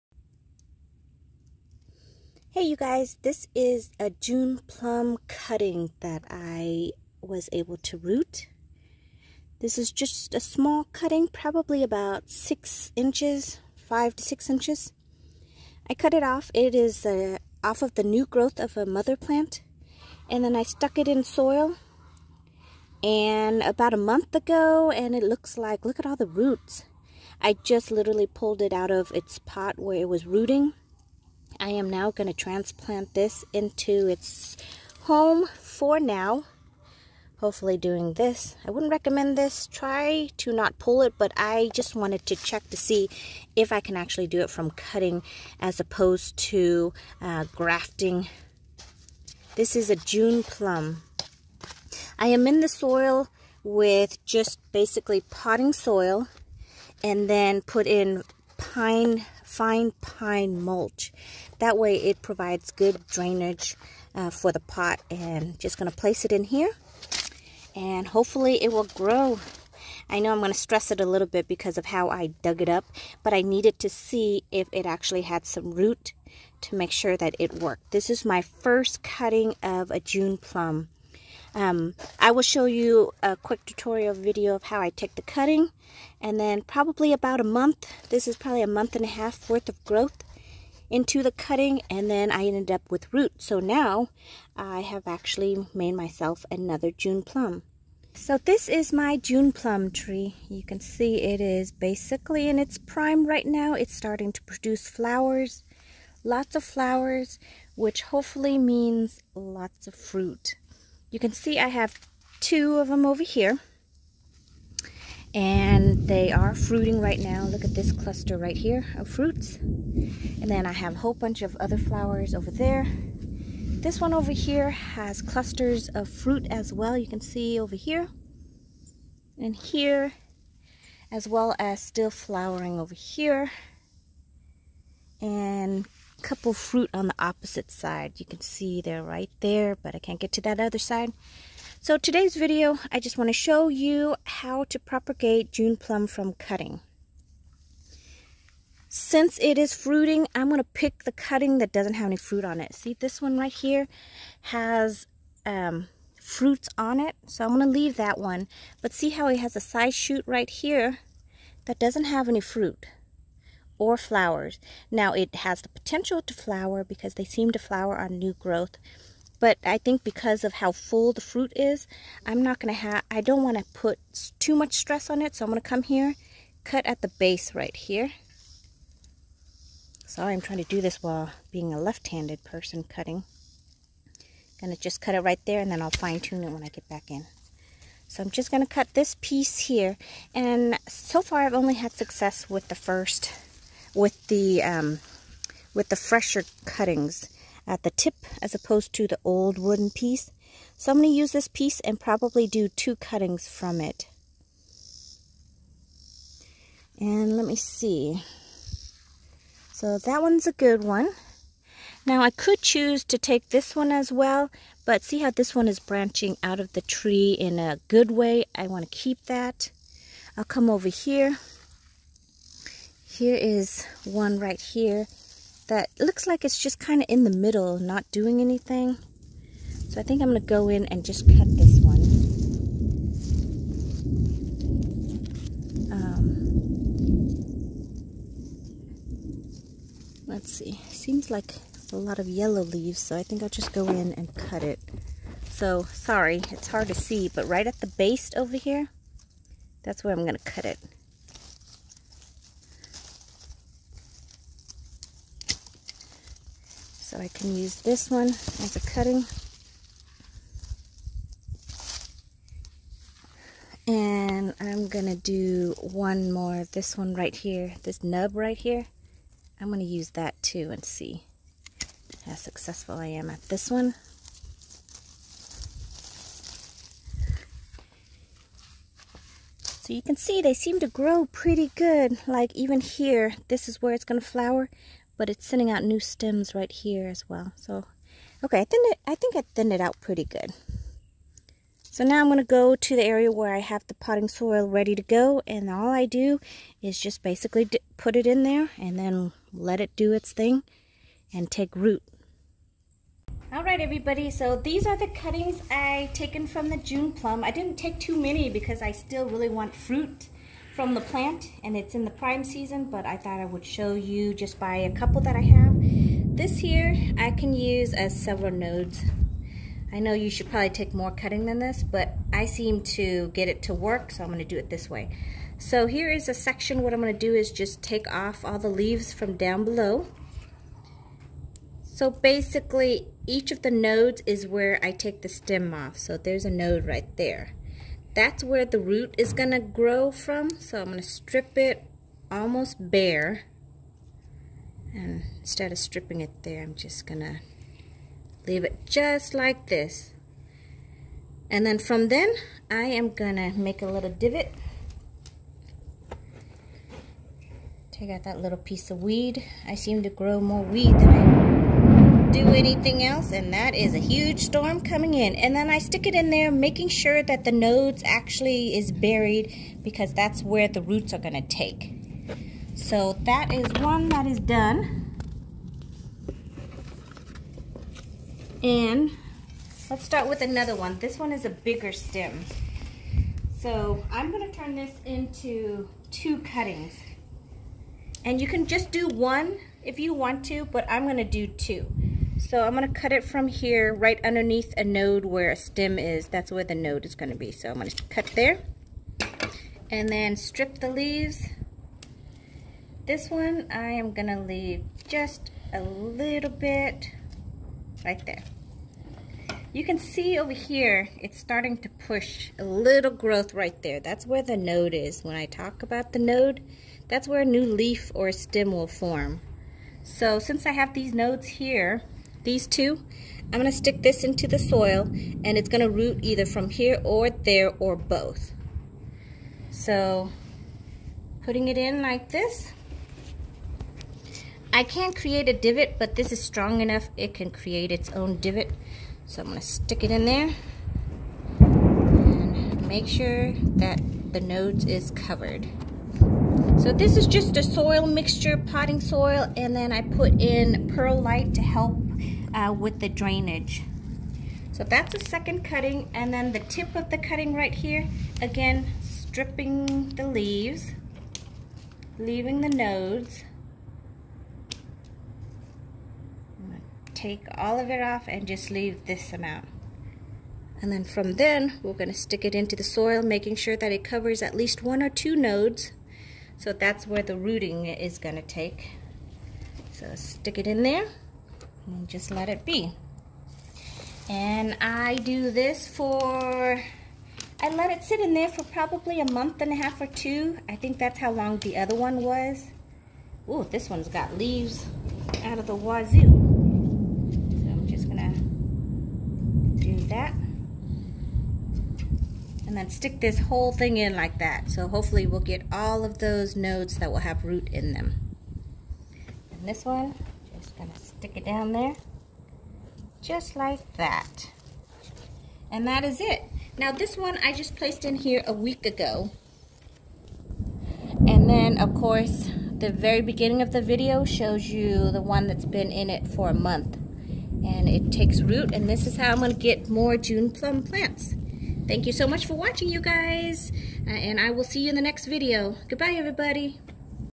How to propagate June plum by cuttings.
oh, this fruit!! !if you have ever tasted it.. you will not forget it.  love tropical fruits.  they bring back childhood memories.  I was able to grow another june plum plant by sticking a cutting into a medium and successfully got roots!.  thank you for watching and I hope this video helps you to create more of the fruit plants you love.